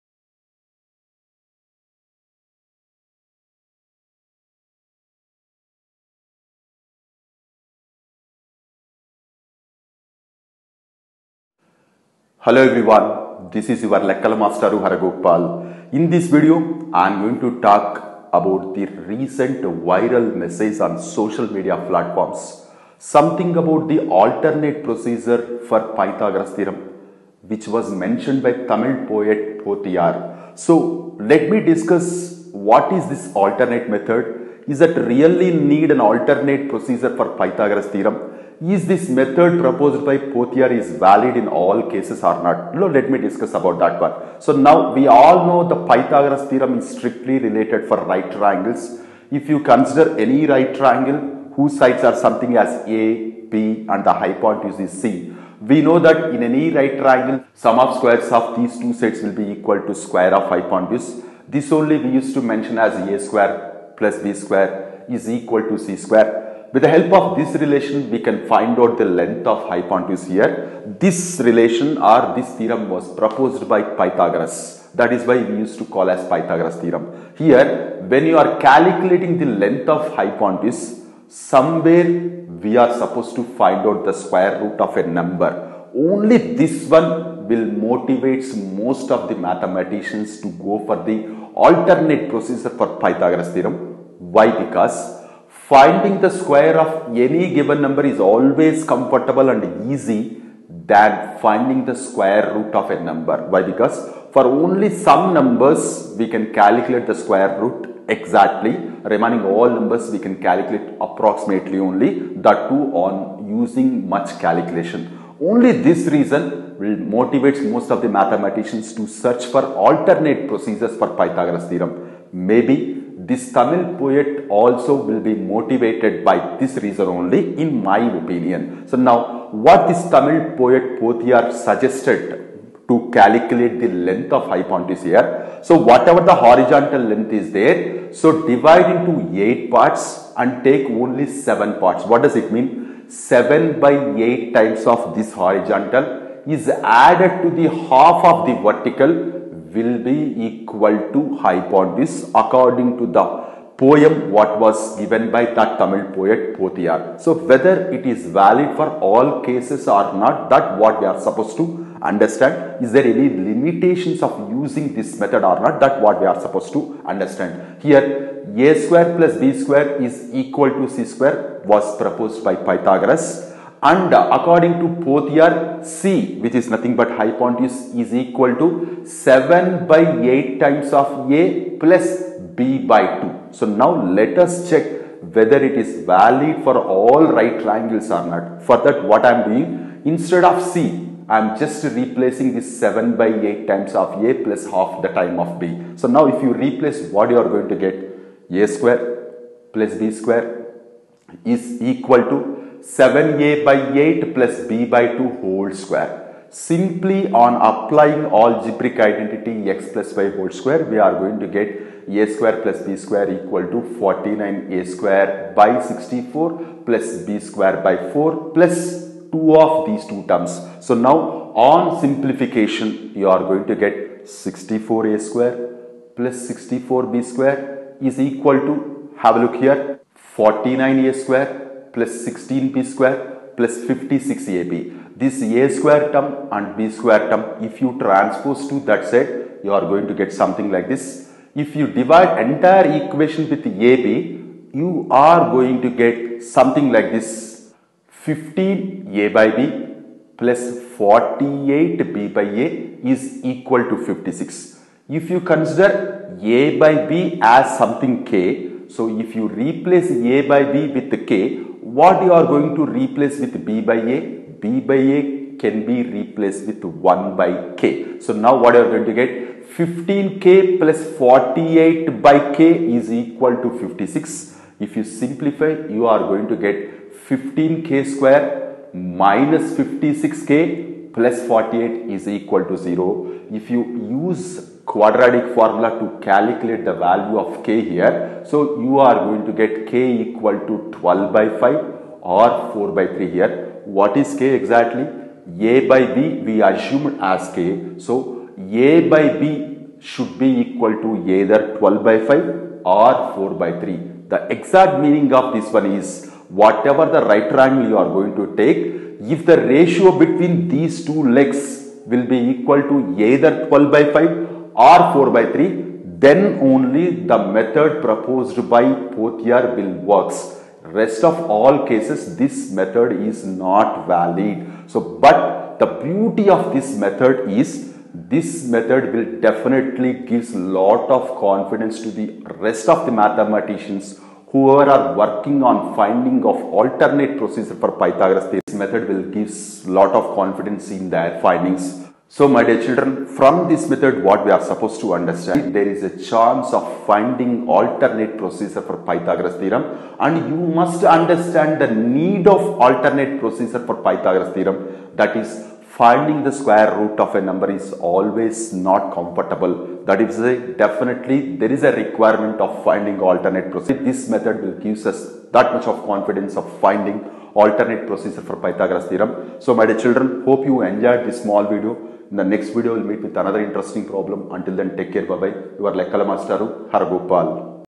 An Alternate for Pythagoras Theorem - Facts & Limitations - LM 450
Pythagoras Theorem
Right Triangle
An Alternate Procedure to Pythagoras
Tamil Poet Bothaiyanaar
Tamil Poet